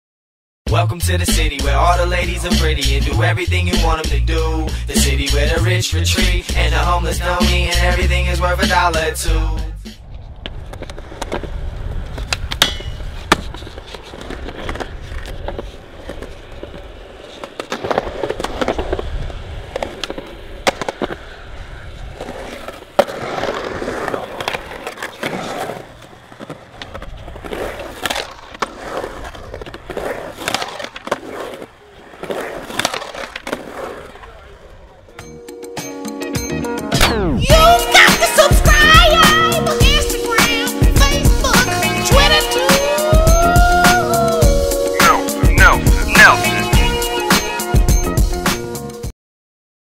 CeeJay Park Shredding
I've never tried front feeble in a bowl but it looks hard!